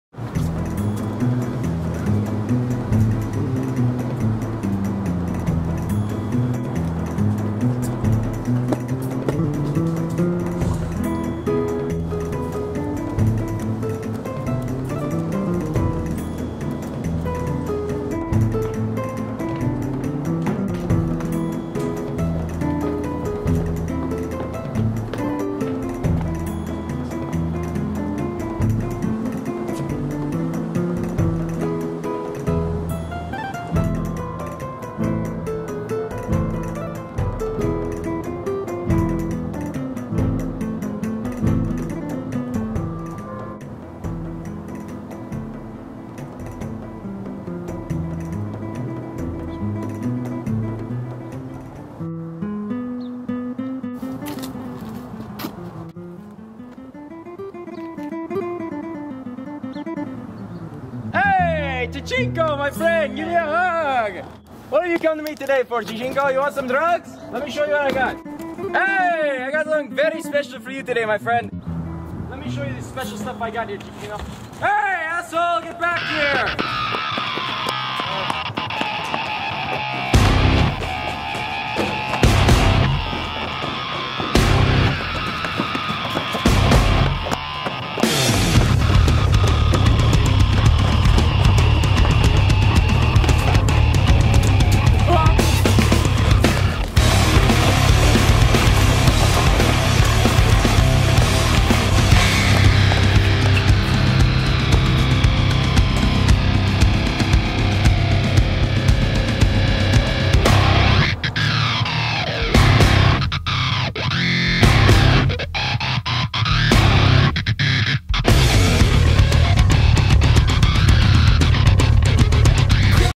Pursuit in Paradise-- Chase Scene #4
For film class we were commissioned to film a chase scene. AlwaysUnscripted broke off into pairs and produced different style chase scenes for y'all. This is number four of the series.